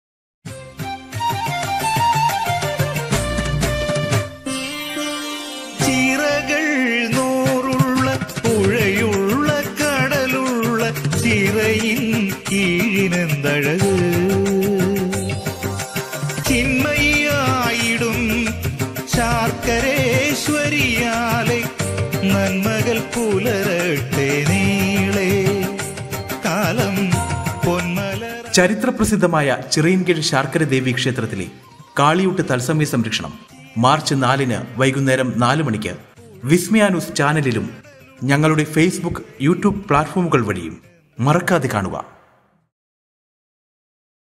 ചിറയിൻകീഴ് ശാർക്കര ദേവി ക്ഷേത്രത്തിലെ കാളിയൂട്ട് മഹോത്സവം തത്സമയ സംപ്രേഷണം മാർച്ച് 4 ന്
ചരിത്ര പ്രസിദ്ധമായ ചിറയിൻകീഴ് ശാർക്കര ദേവി ക്ഷേത്രത്തിലെ കാളിയൂട്ട് മഹോത്സവം
തത്സമയ സംപ്രേഷണം മാർച്ച് 4 ന് വൈകുംനേരം 4 മണി മുതൽ.